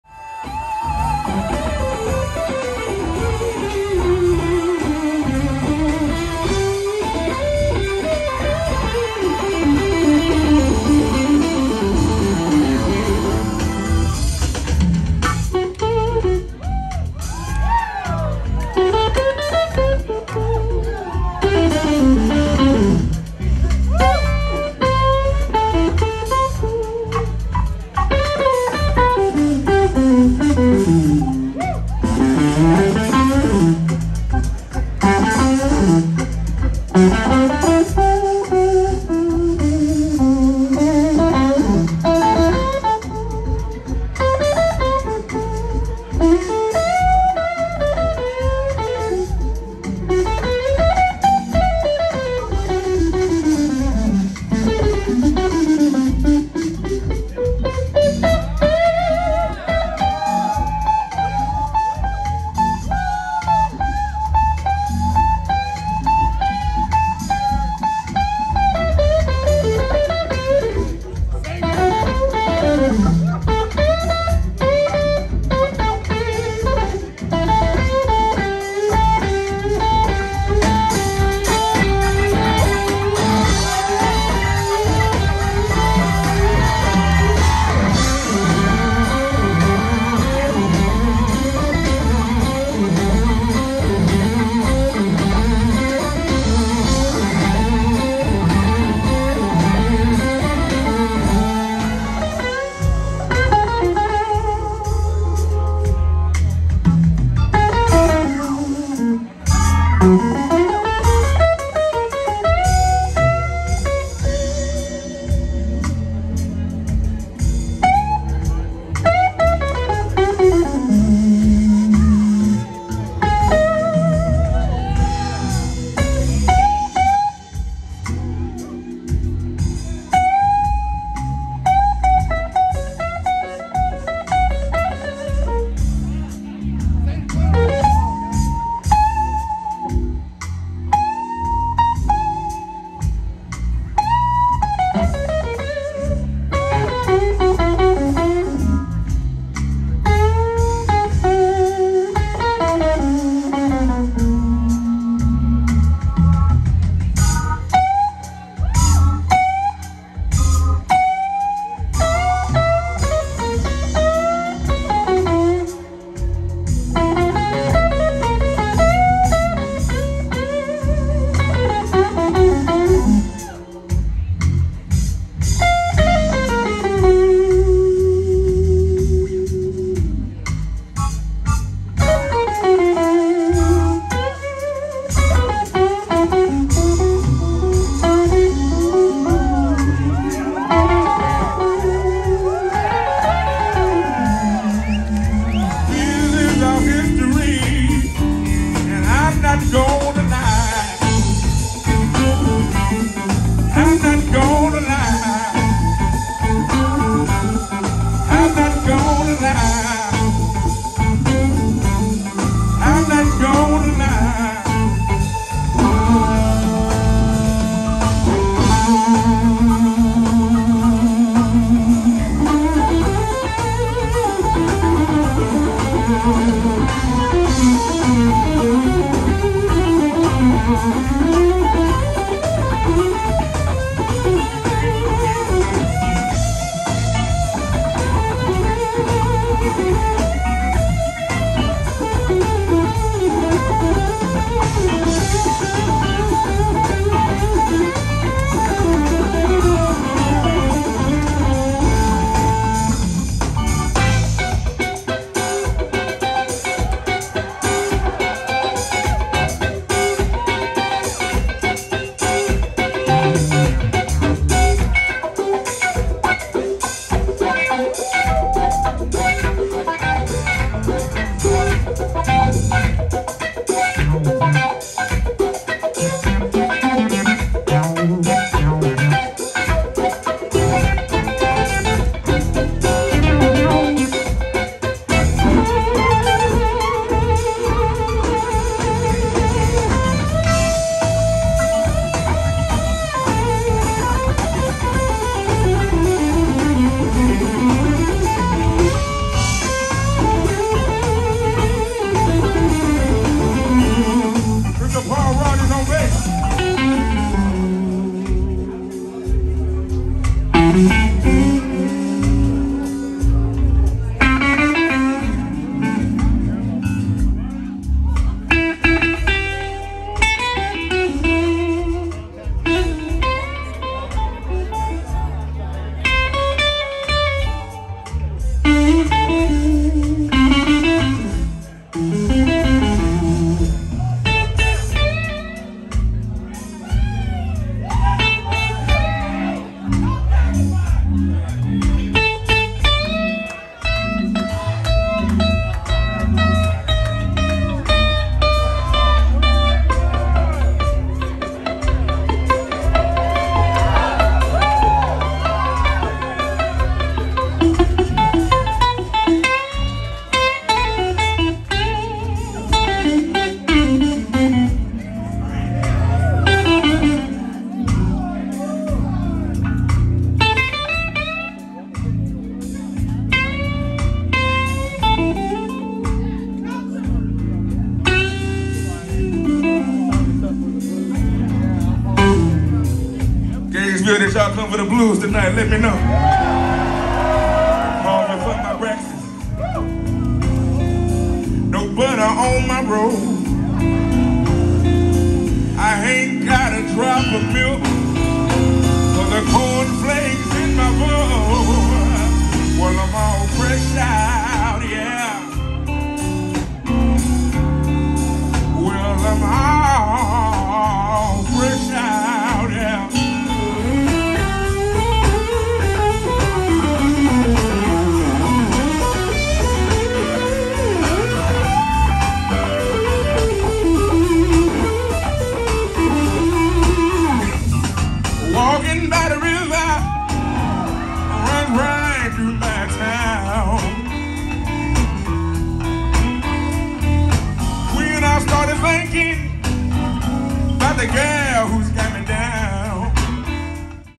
Christone Kingfish Ingram @ the High Dive in Gainesville, Fl
Blues guitarist and legend Christone Kingfish Ingram plays a great show in Gainesville, Fl… here are the highlights.